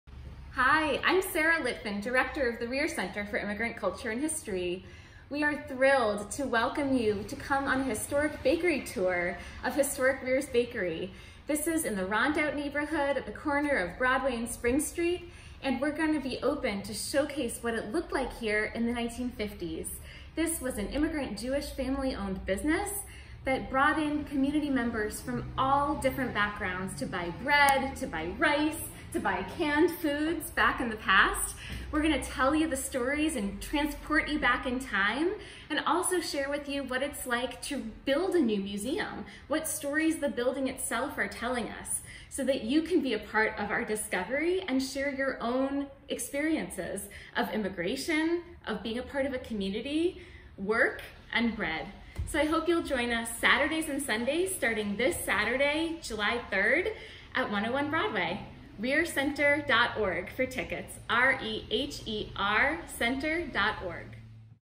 Reher Center in Kingston, NY
Reher Center in Kingston, NY showcases its renovated bakery museum.